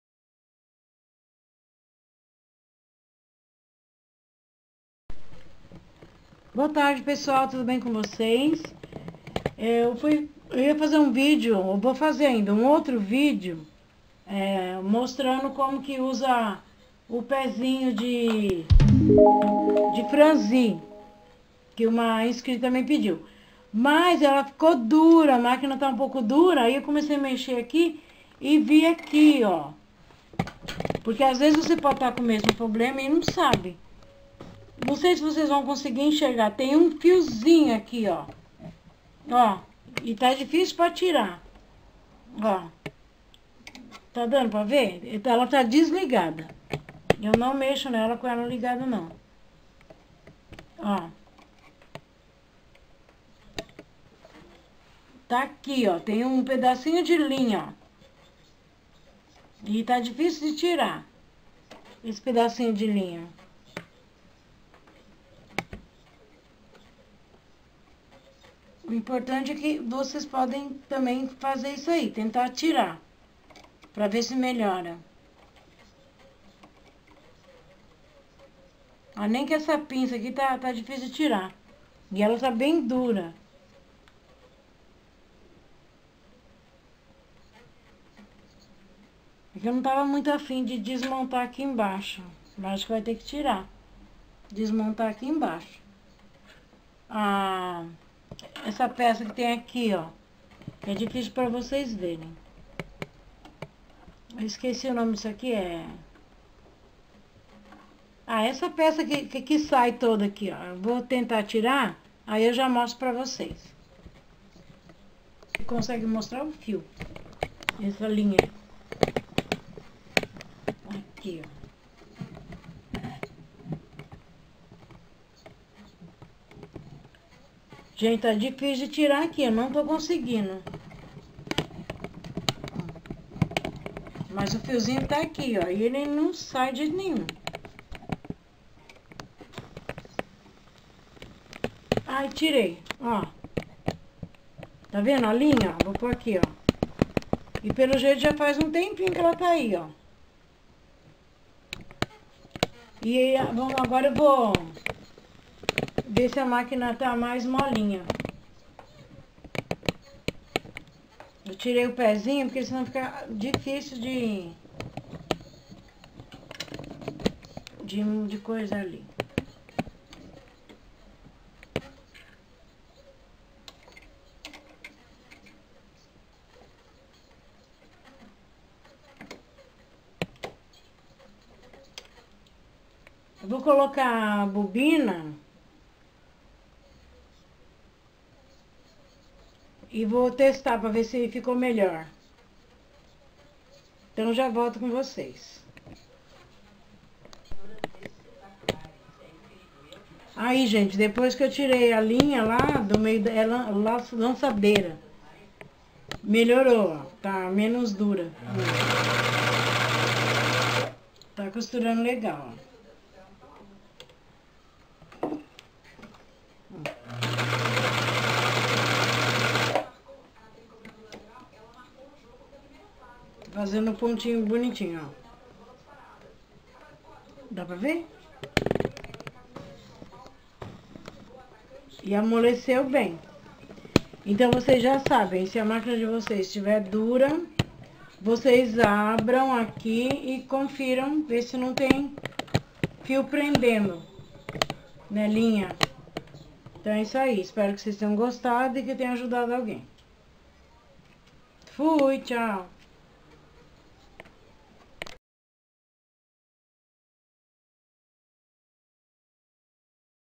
Máquina de Costura Pesada Resolva Você Mesmo
Olá pessoal que ama as máquinas de costura antigas,por causa de uma coisinha a toa minha máquina ficou pesada e dura,um probleminha fácil que você mesmo pode resolver,veja se sua máquina não esta pesada ,dura ou travada por este problema.